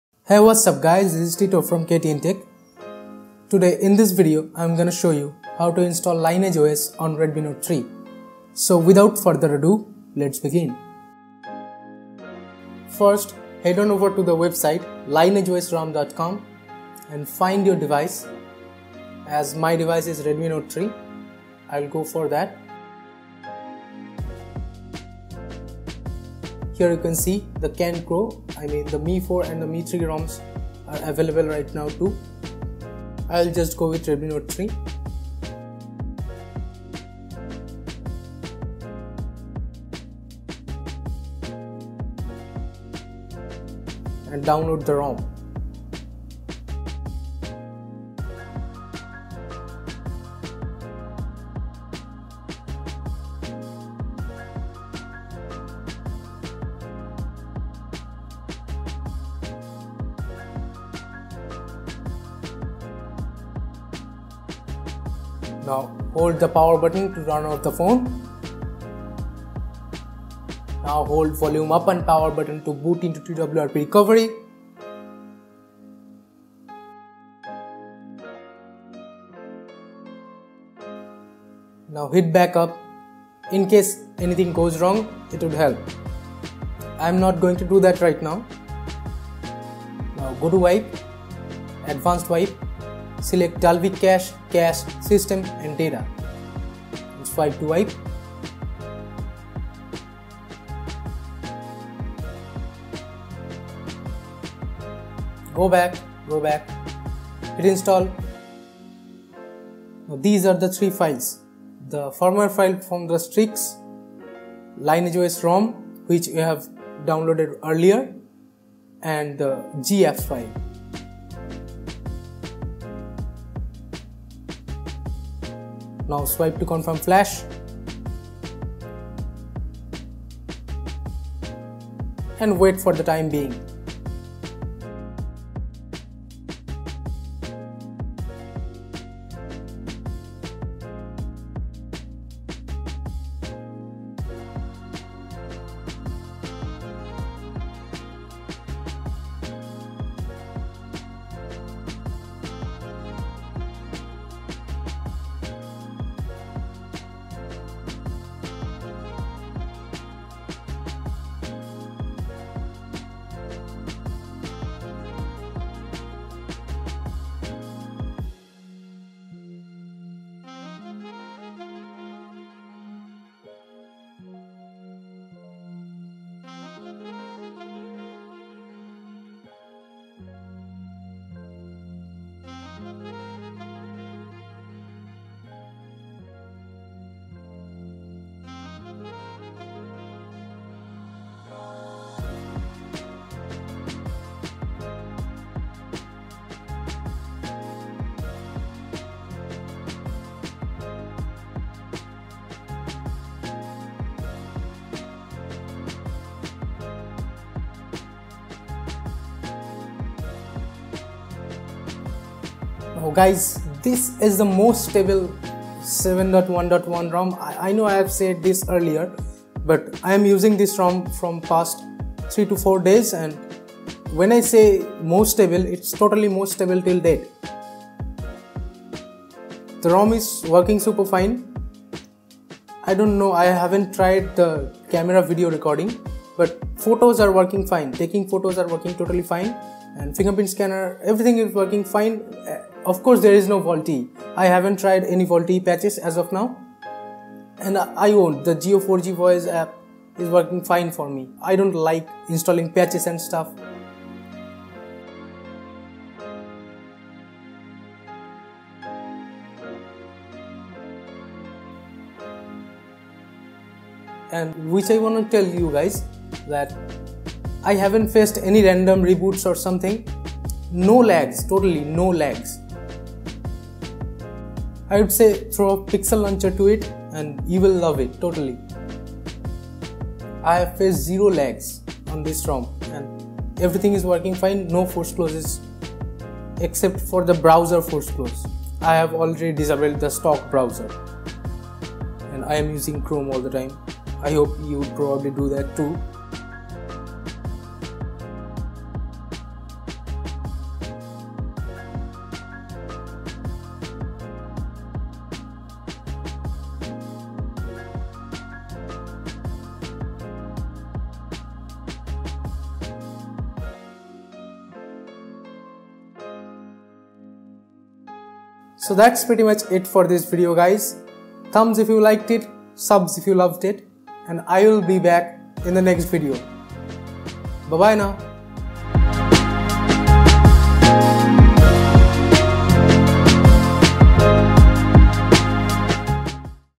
Unofficial LineageOS 7.1.1 on Redmi Note 3 Pro/Kenzo (How to Install)
This is the best way to flash Lineage OS on Redmi Note 3 Pro.

Periscope: @ktntech